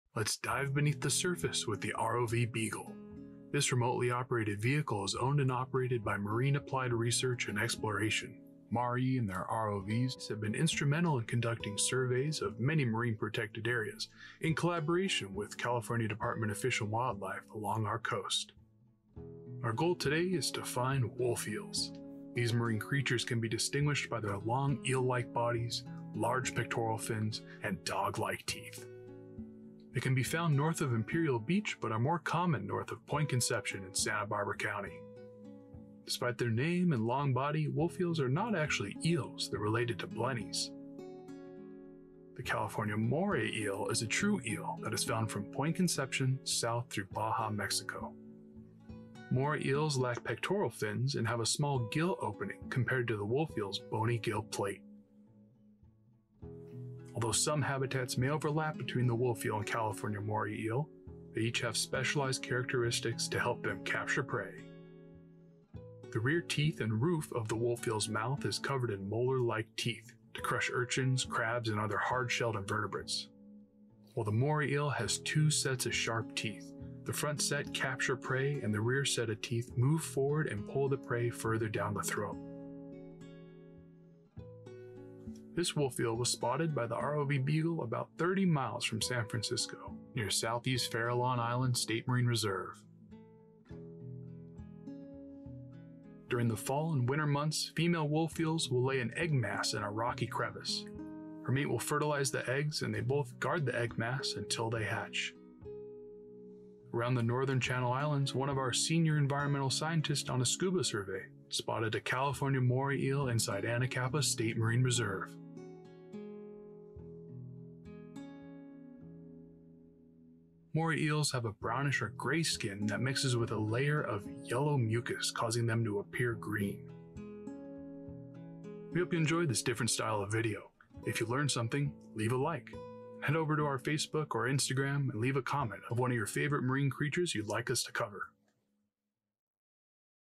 Explore the Differences Between the Wolf Eel and the California Moray Eel
Join the ROV "Beagle" and dive down to the sea floor in search of Wolf eels! This short video highlights some characteristics and behaviors of the Wolf Eel and compares them to the California Moray Eel.